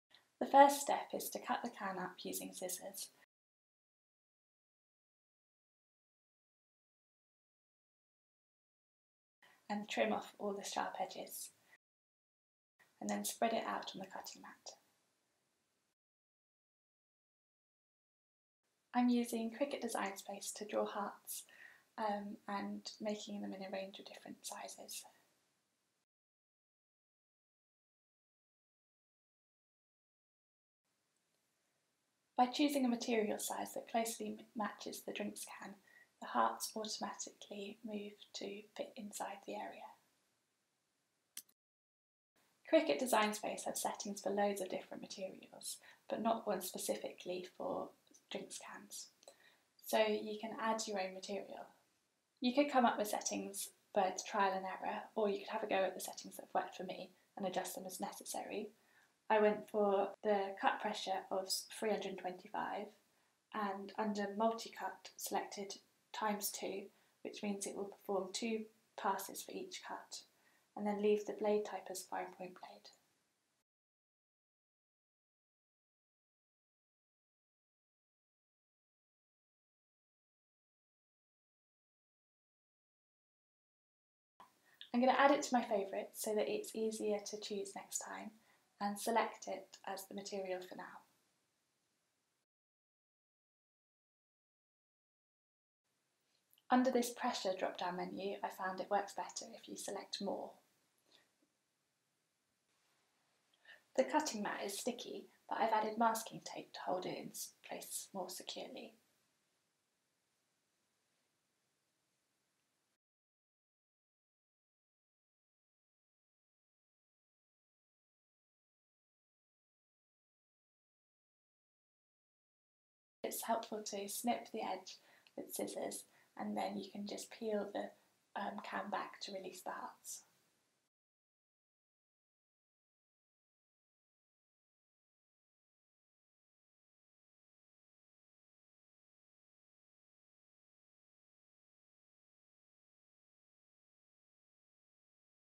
Cut Up Aluminium Drinks Cans for Crafts with a Cricut
In this video I'll show you how easy it is to turn drinks cans into decorative shapes for art projects, using a Cricut® vinyl cutter. For step by step instructions including how to make the card at the end of the video go

If you're looking closely at the video, you may notice a setting for cutting cans before I show how to create one. This is because I practiced doing it before making the video. But it may become a standard material in the software by the time you watch it. In which case, just miss out that step.

Safety warning: Be careful of sharp edges